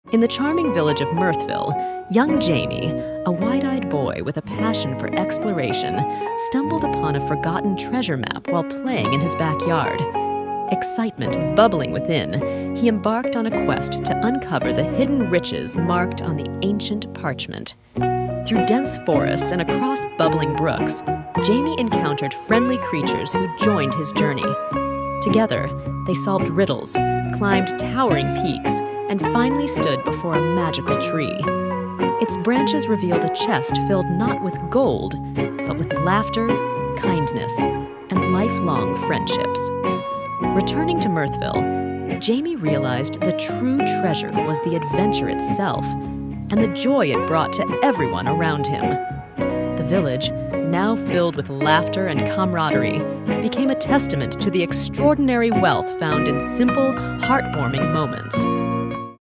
Jamie's Quest for Joy : The Treasure Within Mirthville
Embark on a heartwarming journey in 'Jamie's Quest for Joy: The Treasure Within Mirthville.' In the quaint village of Mirthville, young Jamie, a wide-eyed adventurer, stumbles upon an ancient treasure map during a playful day in his backyard. Fueled by excitement, he sets off on a quest through dense forests and bubbling brooks, accompanied by friendly creatures.

Together, Jamie and his newfound friends solve riddles, climb towering peaks, and discover a magical tree whose branches hold a different kind of treasure—laughter, kindness, and lifelong friendships. Returning to Mirthville, Jamie realizes that the true riches lie not in material wealth but in the adventure itself and the joy it brings.

Witness the transformation of Mirthville into a village filled with laughter and camaraderie, a testament to the extraordinary wealth found in the simple, heartwarming moments of life. Join Jamie in uncovering the hidden treasures that truly matter in this enchanting tale of discovery and friendship."